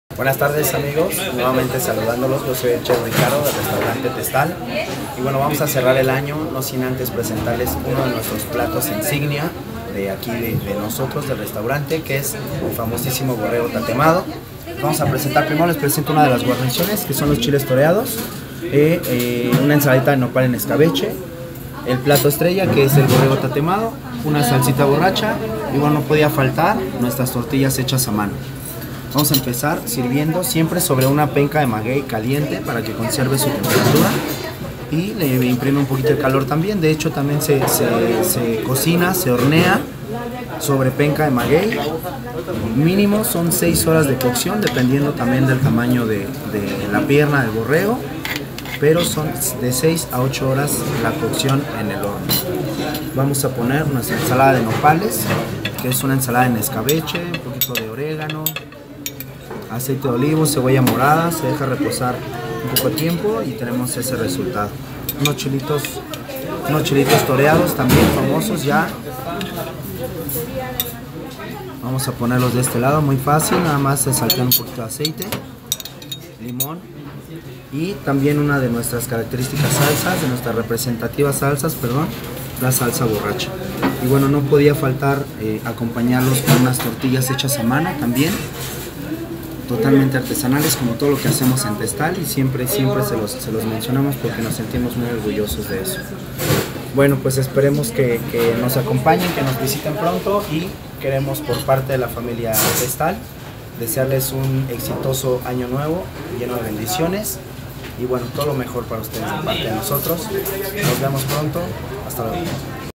Borrego TESTAL.
Horneado lentamente en penca de Maguey, adobo y Chiles secos.
¿Se te antoja? 😋